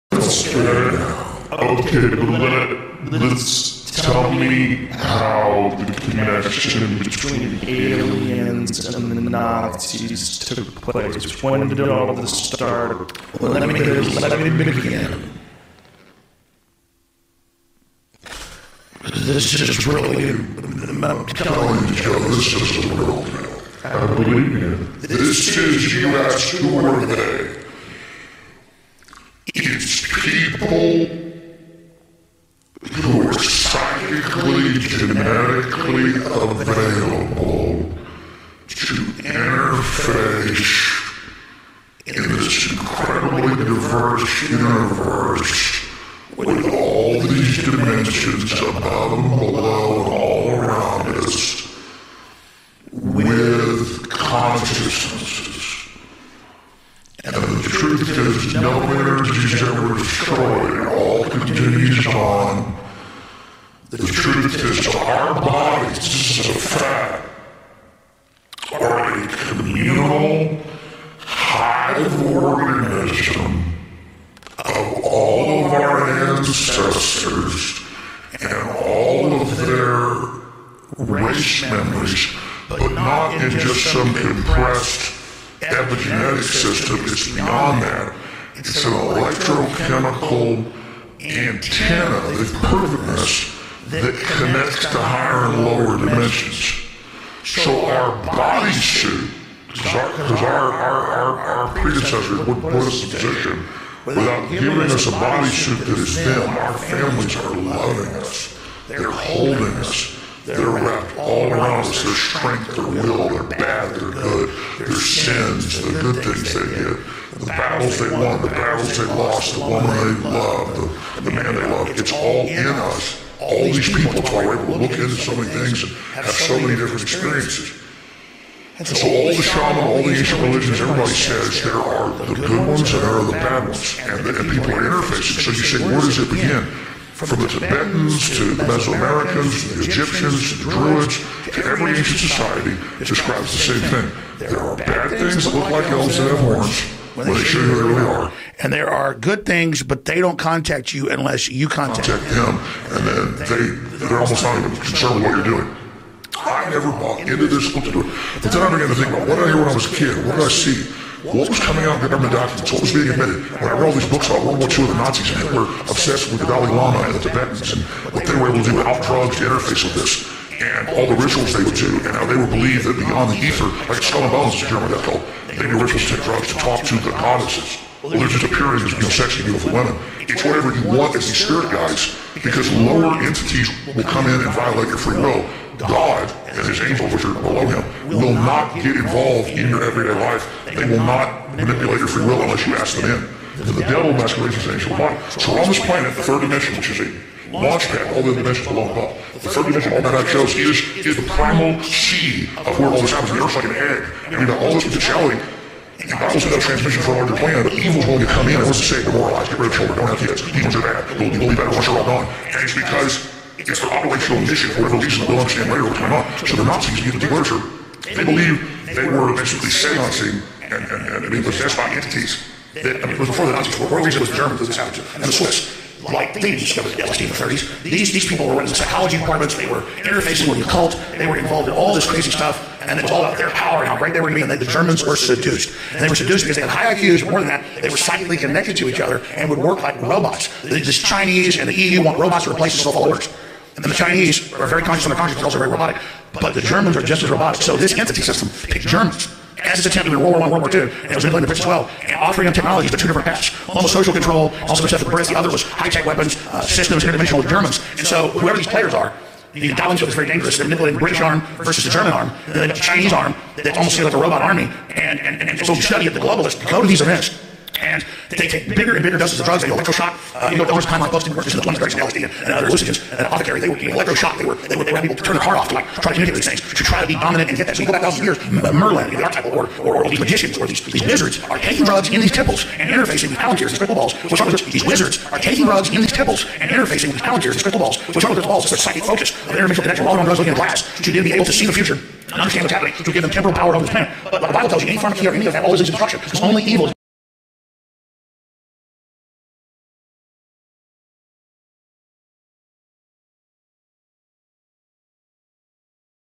THE ALEX ROGAN PANDEMONIUM SHADOW SHOW
If Infowars is the victim of a KILLSWITCH..why is it still streaming on ITUNES? (APPLE INC OWNED)   LOL

This called PIVOTING in the business world.